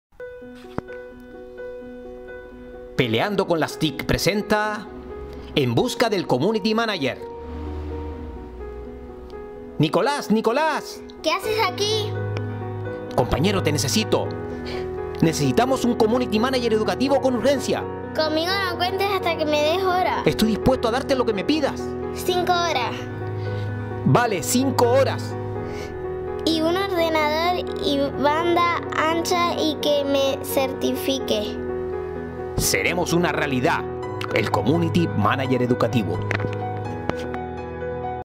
Campaña publicitaria "ficticia" Community Manager Educativo
En el cole estamos buscando un community manager educativo. Nadie quiere ser y hemos lanzado esta campaña en los medios. ¿Se apuntará alguien? ¡Con uno nos conformamos!